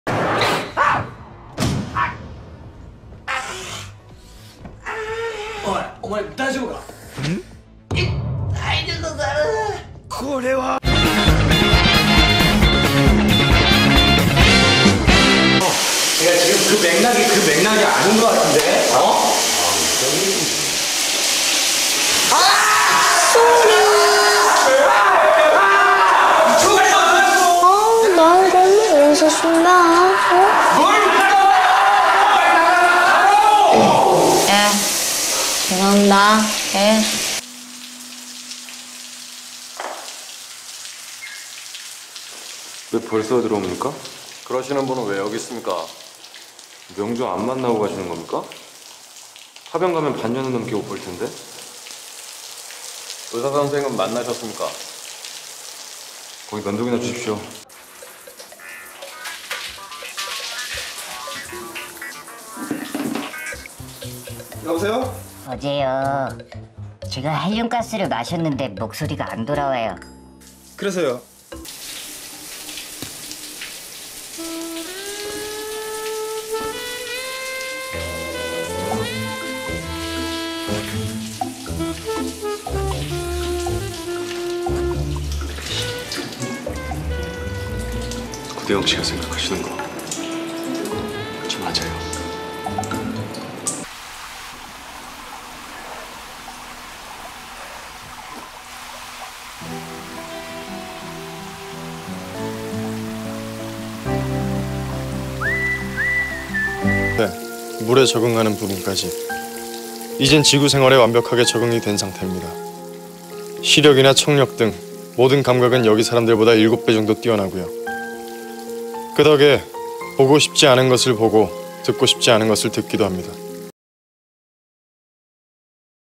濕身淋浴超肌情！你被哪些韓劇歐爸誘惑了？
濕身歐爸：宋仲基、鄭珍雲、‎尹斗俊、蘇志燮、金秀賢